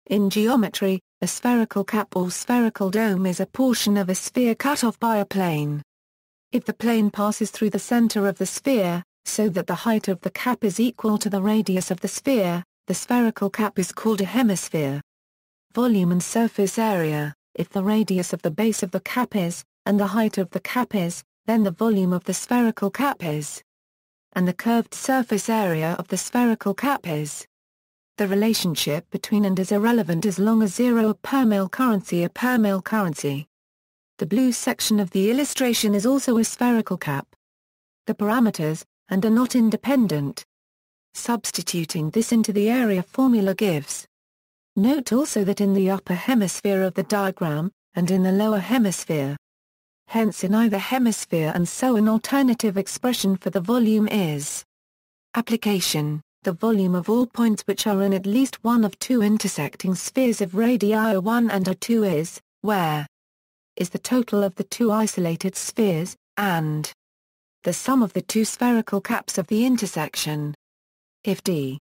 Spherical cap
In geometry, a spherical cap or spherical dome is a portion of a sphere cut off by a plane. If the plane passes through the center of the sphere, so that the height of the cap is equal to the radius of the sphere, the spherical cap is called a hemisphere.